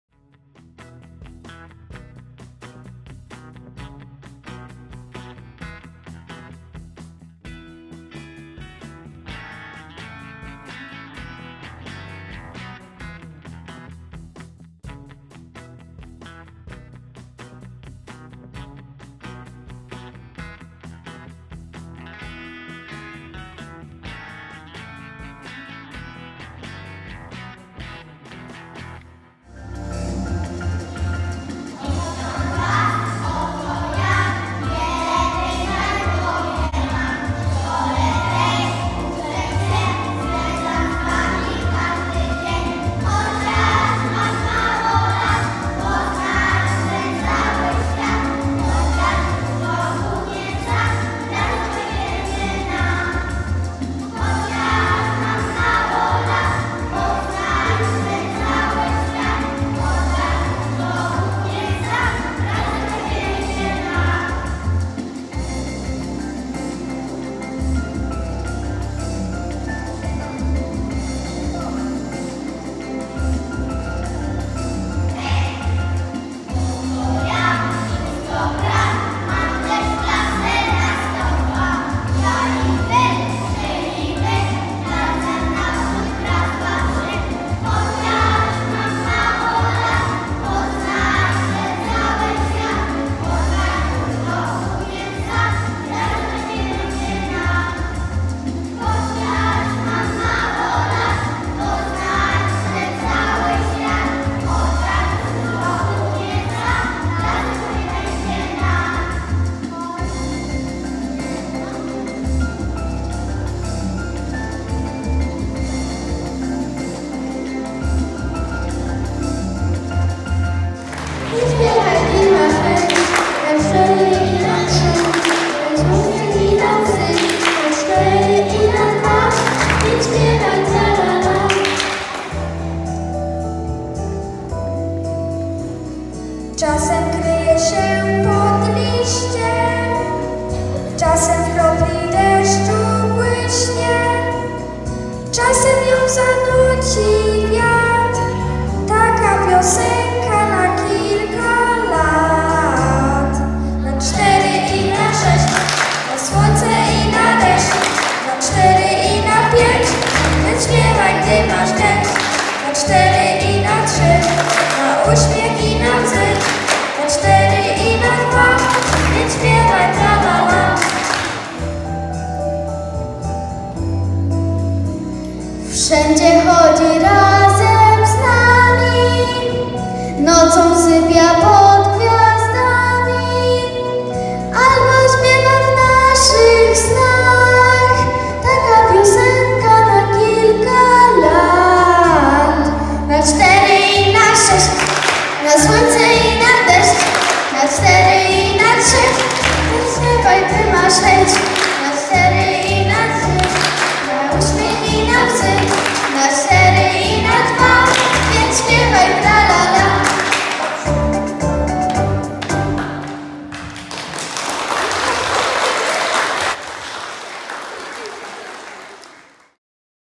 Pasowanie uczniów klasy I -2019-
Videorelacja z pasowania uczniów klasy I.
Uroczystość odbyła się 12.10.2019 r.
Zdjęcia, video, montaż: Sylwiusz Kuliczkowski